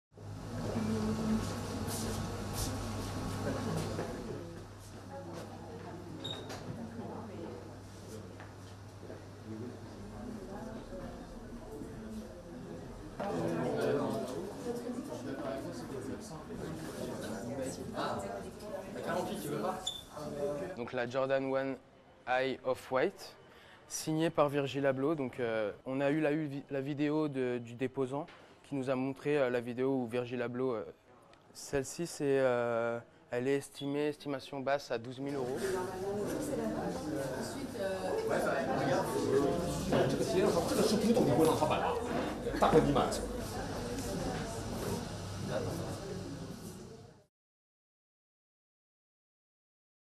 أحذية رياضية للبيع بالمزاد في باريس
بيع أكثر من 200 زوج من الأحذية الرياضية في باريس، من نموذج موقّع يدوياً من قبل فيرجيل أبلوه